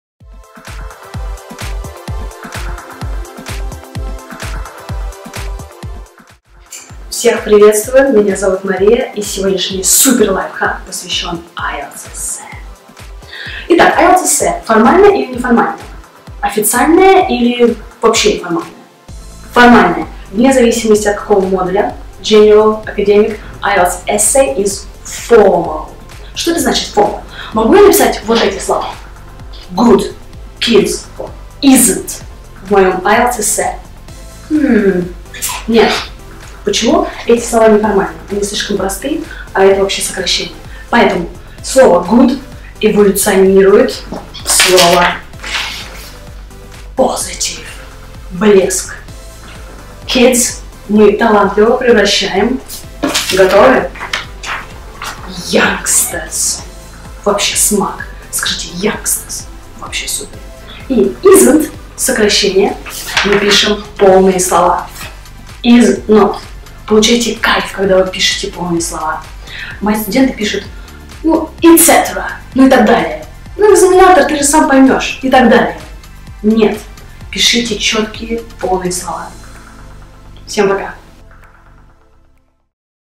Экзамен IELTS. Как сдать успешно?
Экзамен IELTS. Как успешно сдать экзамен расскажет Мария, преподаватель школы BKC-IH